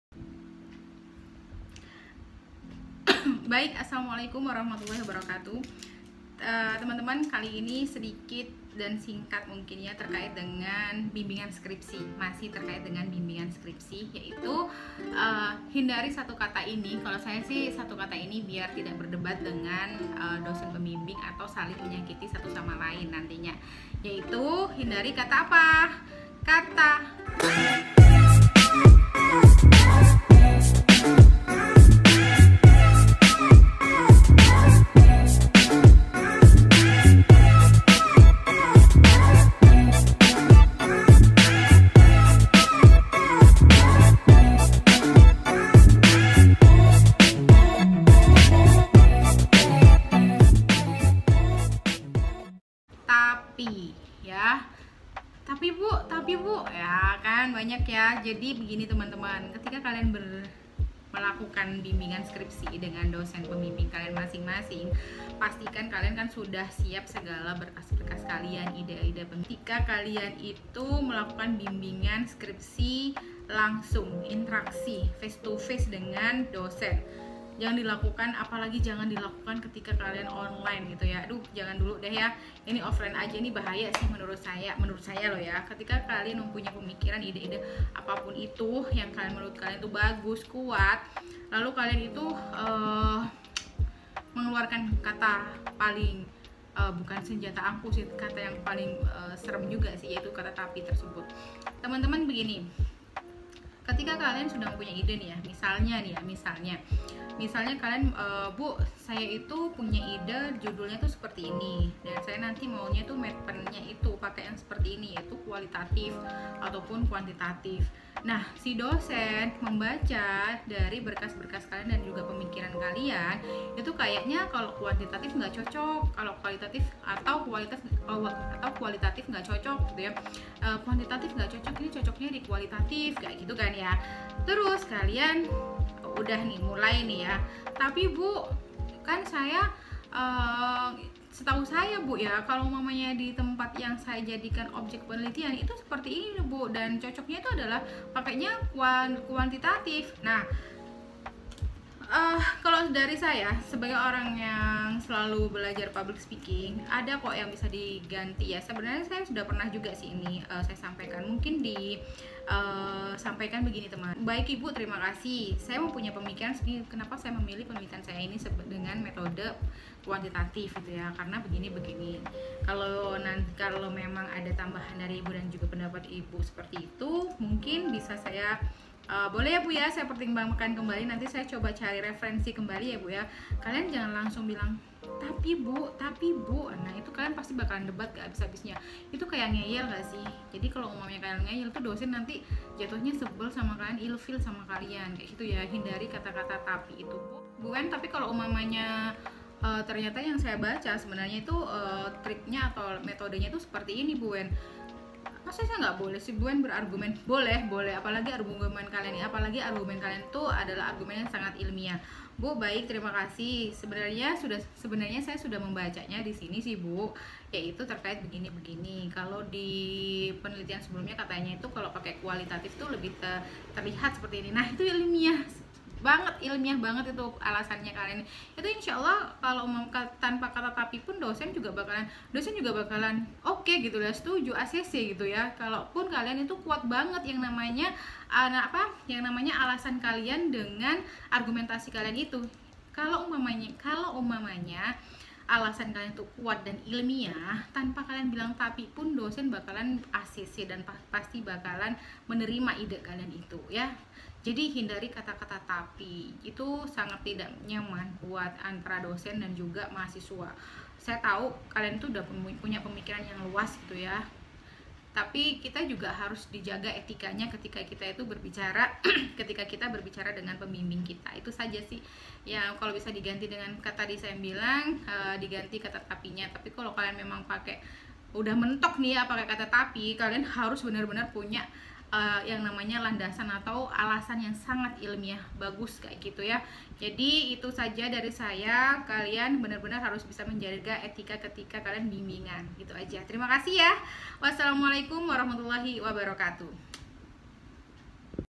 HINDARI SATU KATA INI KETIKA BIMBINGAN SKRIPSI
Assalamualaikum

Yeyy ada tema unik lagi nih di dunia perskripsian

Yaitu menghindari kata kata yang kurang berkenan yess salah satunya kata ini yaaa

Yuk siman

Terimakasih
Wassalamualaikum